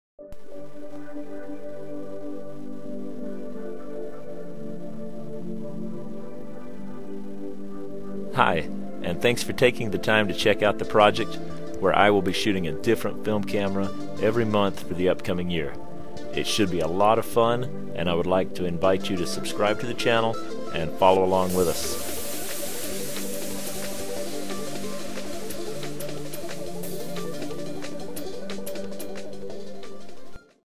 Channel Introduction 12 Months 12 Cameras Film Project
Intro to the photography project where I will shoot a different film camera every month for the upcoming year. I'll give a brief overview of the camera, take the viewers along while I shoot the camera, share images, and end with my thoughts on the camera. If you are into film, or just considering it, this should be an interesting project.

*All video, graphics, and images are mine